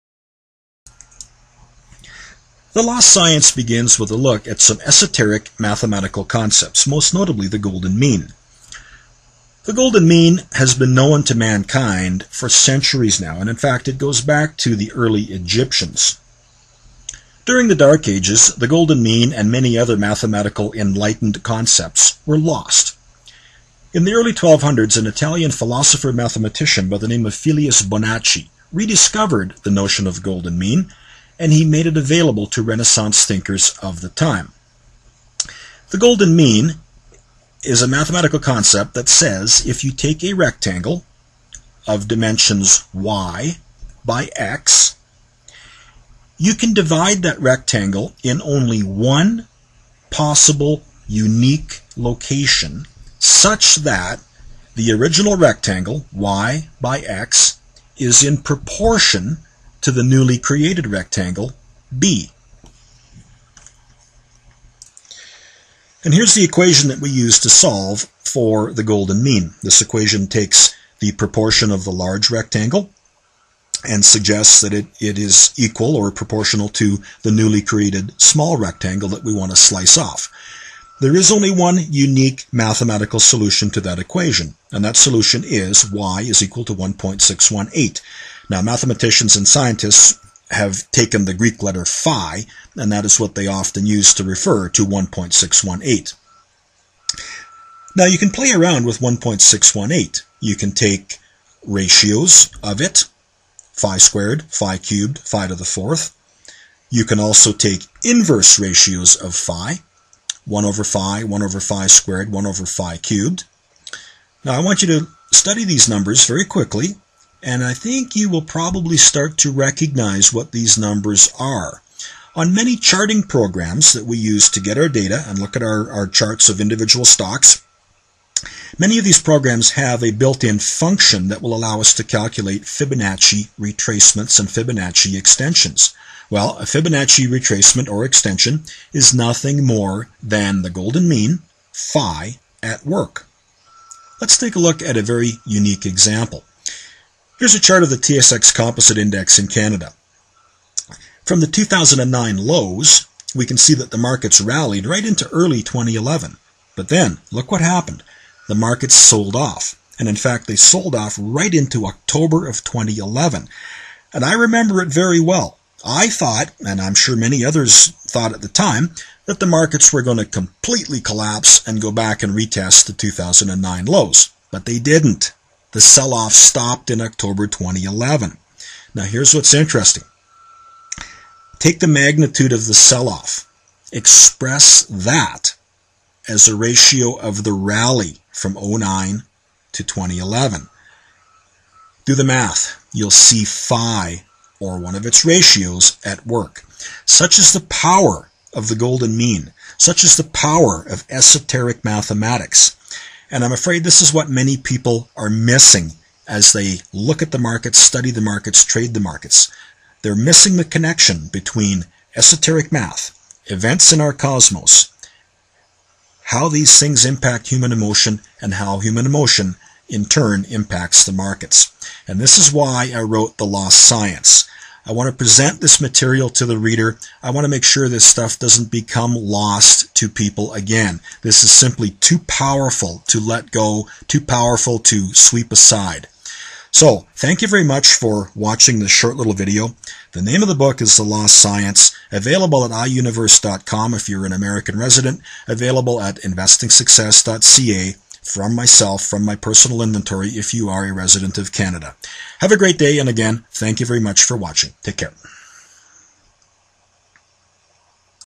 Esoteric Math and the Markets
1.618, also called phi or the Golden Mean is a powerful mathematical concept with profound implications for traders and investors. This brief video clips shows you the power of 1.618.